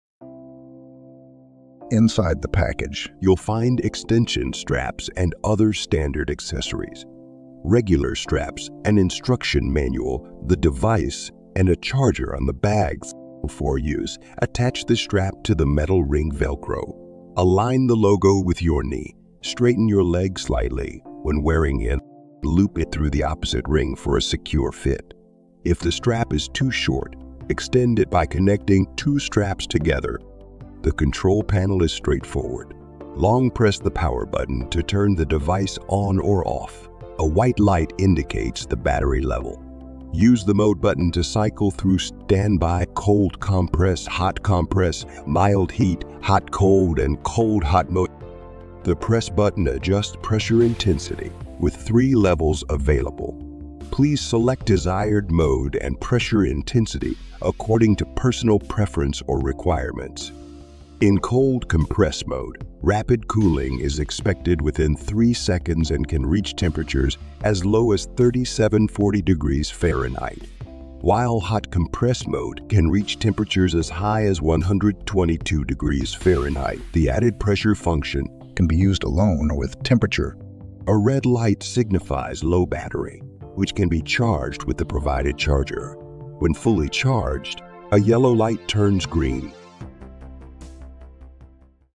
Knee and Leg Massagers with Ice and Heated Therapy instructions
The knee and leg massagers use ice and heat therapy and are a multifunctional device designed to relieve pain, improve circulation, and relax muscles. Here is a comprehensive guide on how to effectively use these devices